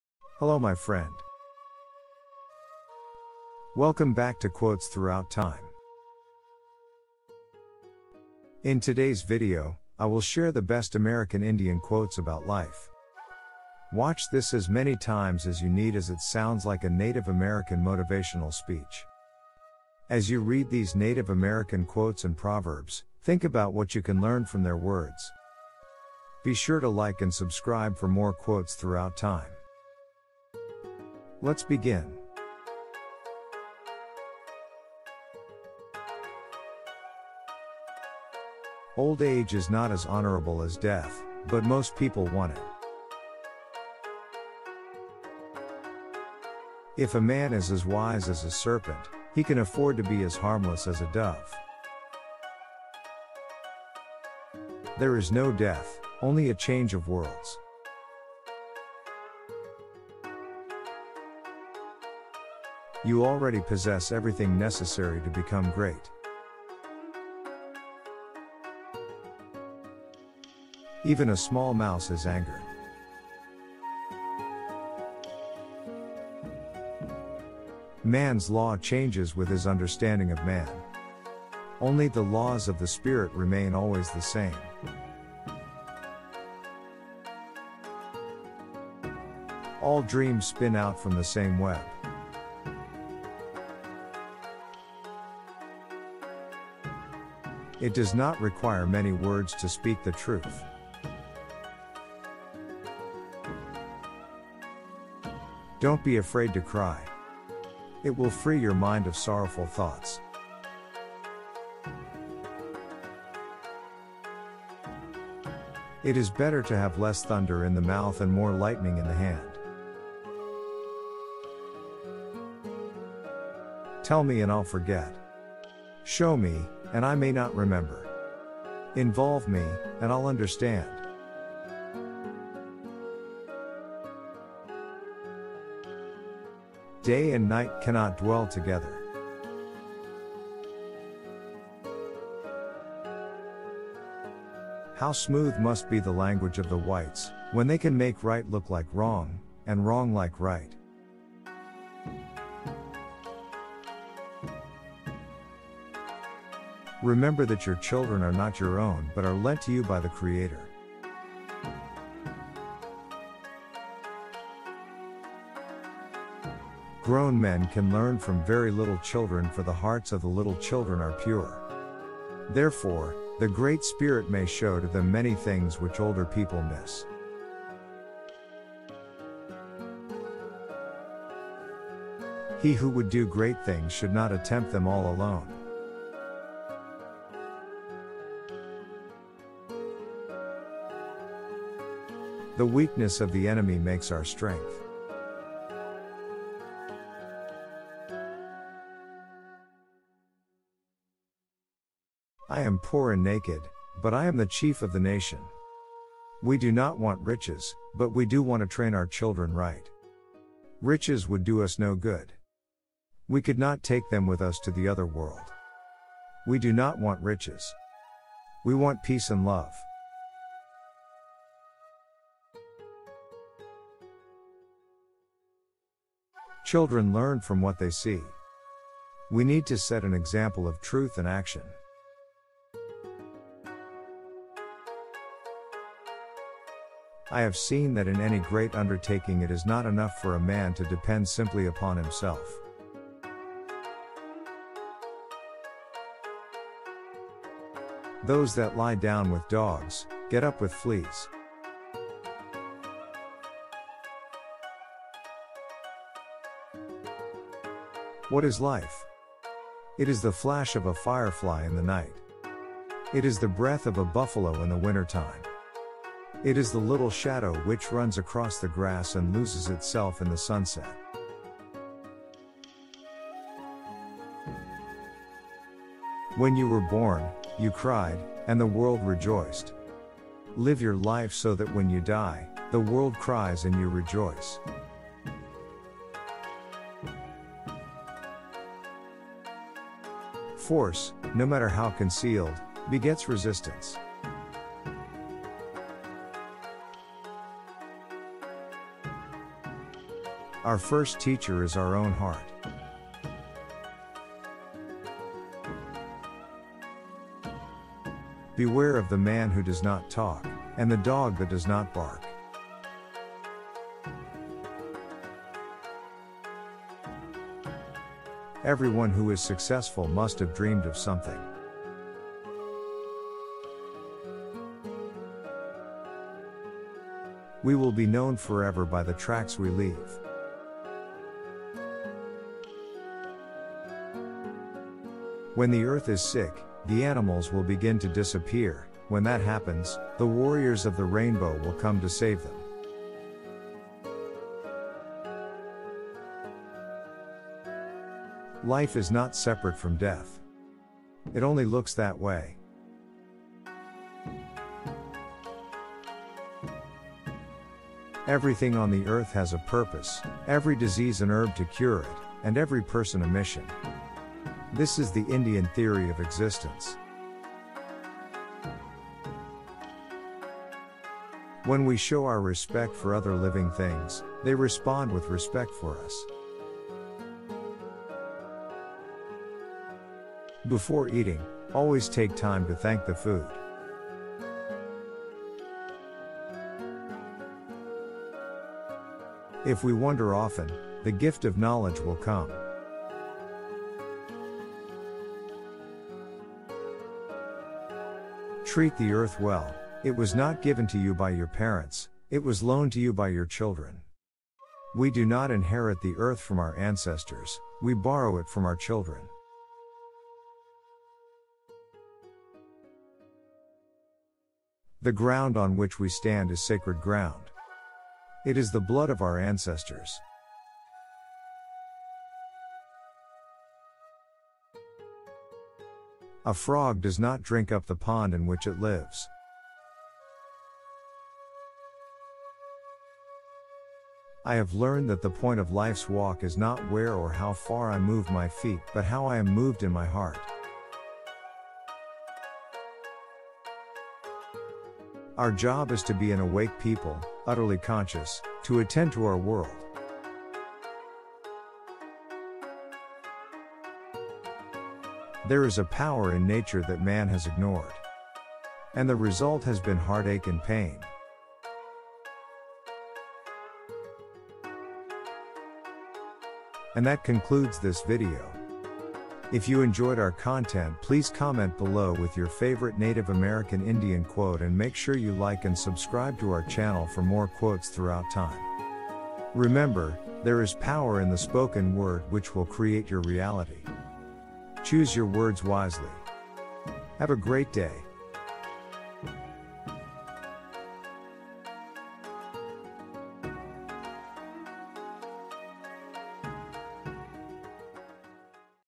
American Indian Quotes About Life | Native American Motivational Speech
In today’s video, I will share the best American Indian Quotes About Life. Watch this as many times as you need as it sounds like a Native American Motivational Speech.
As you read these Native American quotes and proverbs, think about what you can learn from their words.